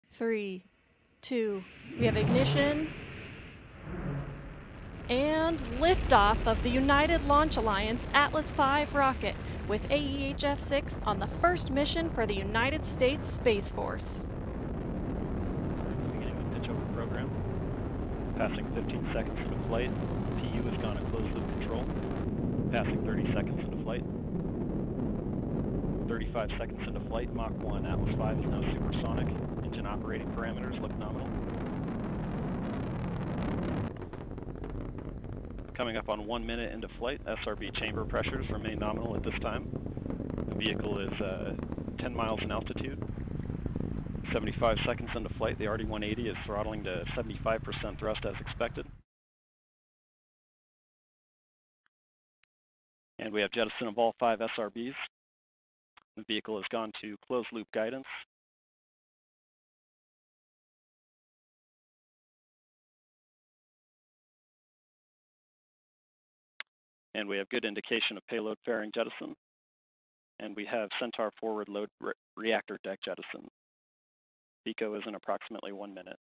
U.S. Space Force takes off on first mission
The U.S. Space Force embarks on its first mission launching a communications satellite into space from Cape Canaveral, Florida.